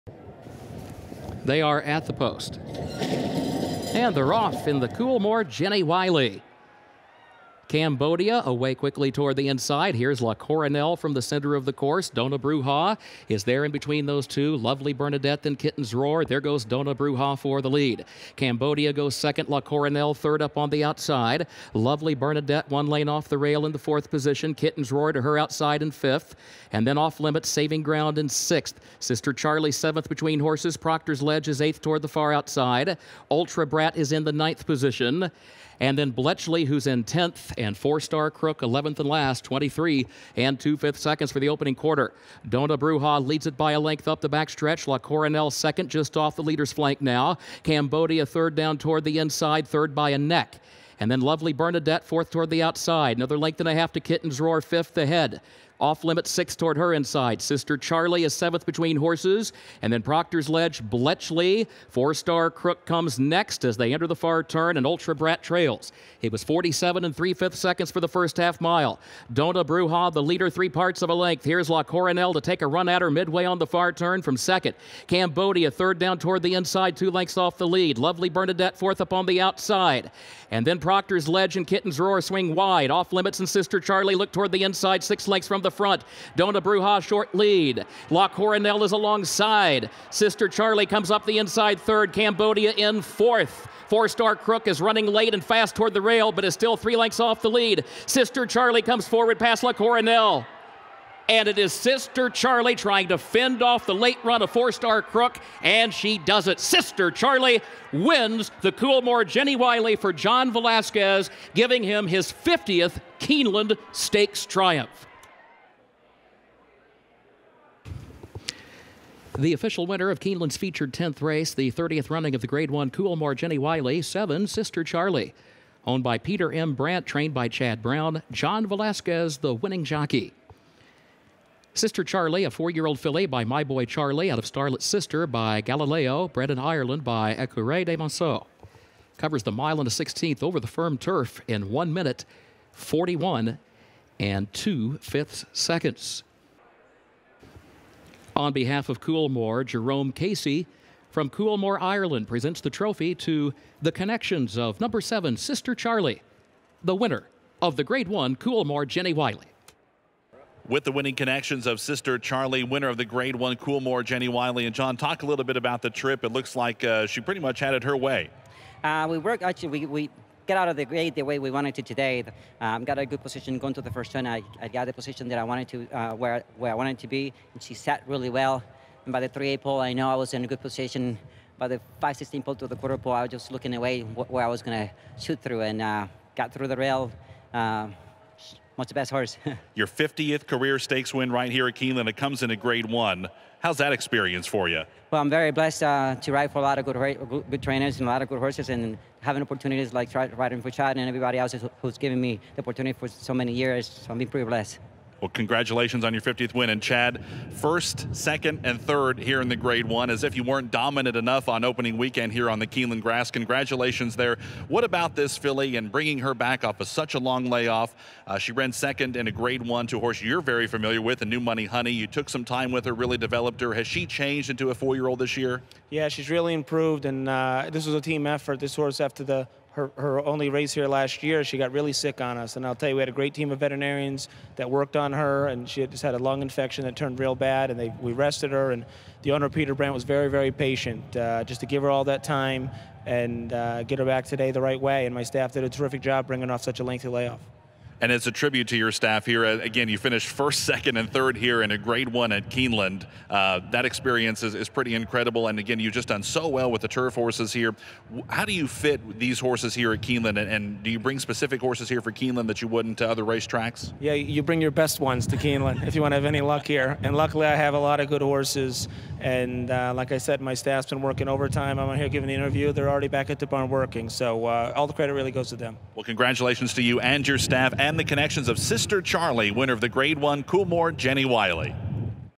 2018 Coolmore Jenny Wiley (G1) - SISTERCHARLIE (IRE)
WATCH → the replay of Sistercharlie (IRE) leading home a Chad Brown trifecta in the Coolmore Jenny Wiley